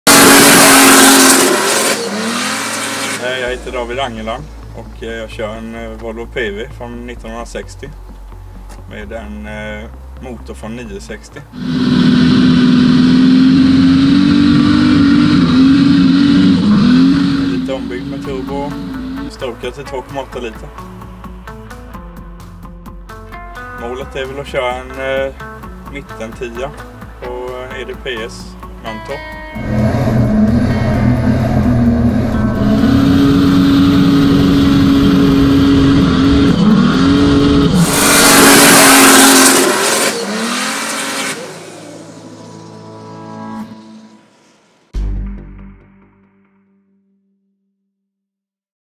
Volvo PV 544 med Volvo 960-motor till EDPS Mantorp Park
Volvo PV 544 - med vässad motor från en 960! På lördag ska David Angeland försöka klämma en mitten-10:a på EDPS Mantorp Park. Åk dit och se om han lyckas!